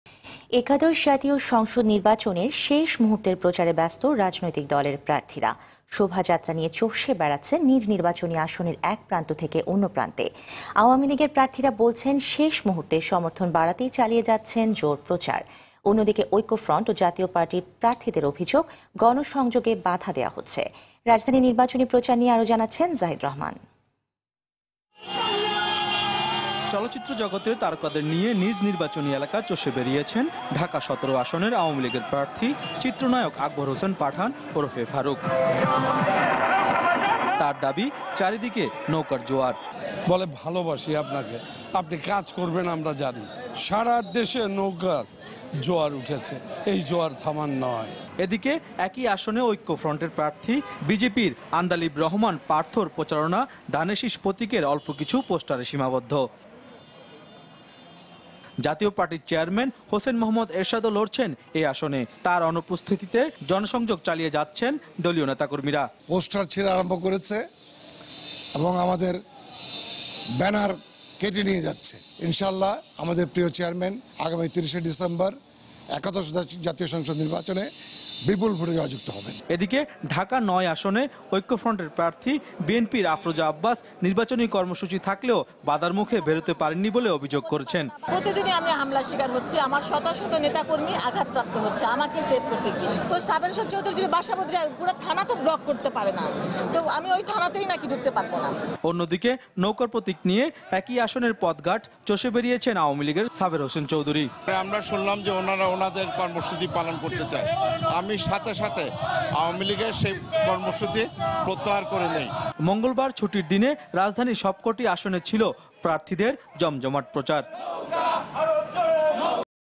একাদশ জাতীয় সংসদ নির্বাচনের শেষ মুহুর্তের প্রচারে ব্যস্ত, রাজনৈতিক দলের প্রার্থীরা
News : একাদশ জাতীয় সংসদ নির্বাচনের শেষ মুহুর্তের প্রচারে ব্যস্ত, রাজনৈতিক দলের প্রার্থীরা

- Team RTV